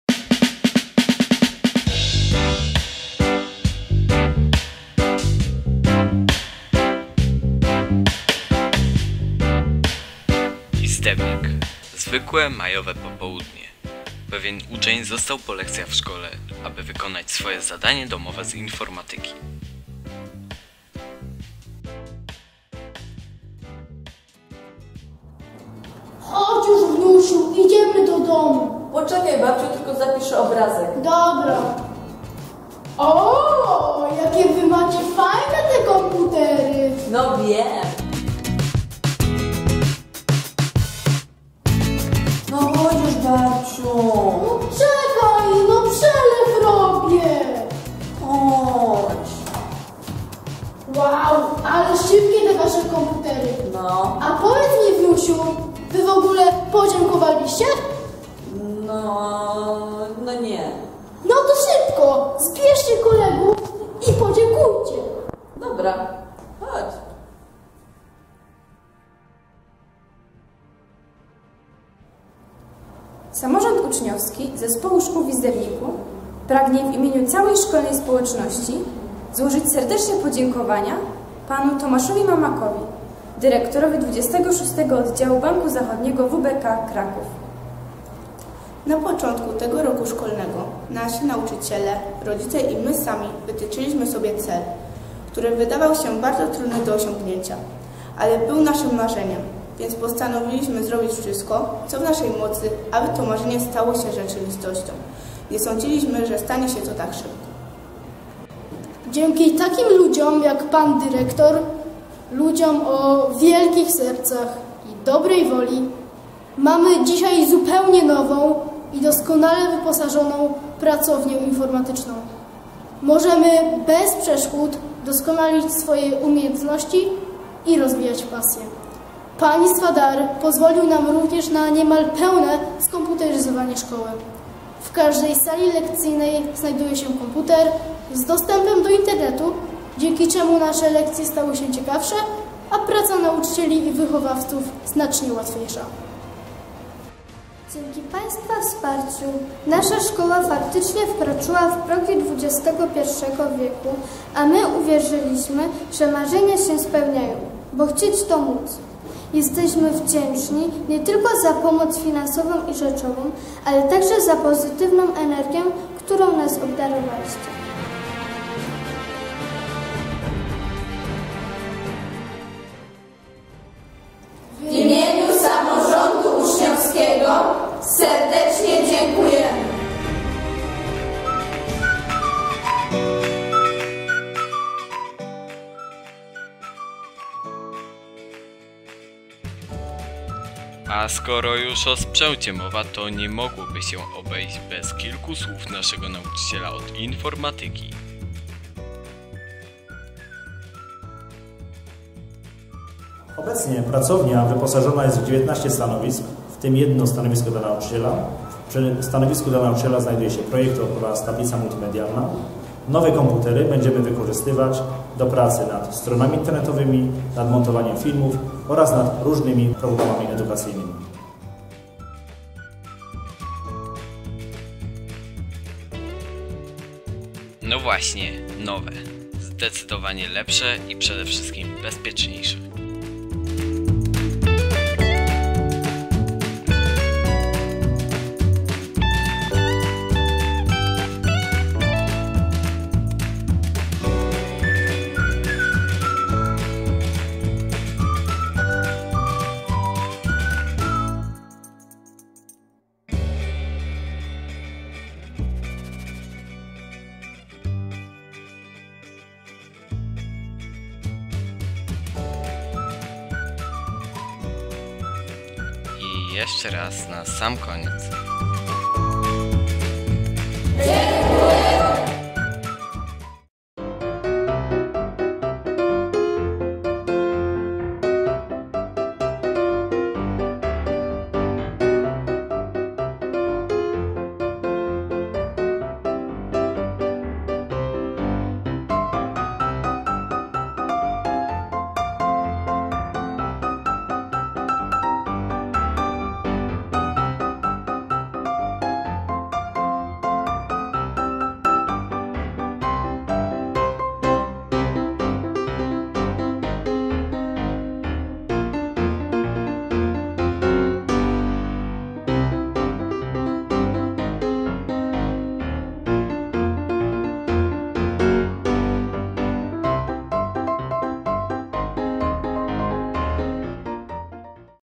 Podziękowania dla Banku Zachodniego WBK
Podziękowania za darowiznę dla naszej szkoły :)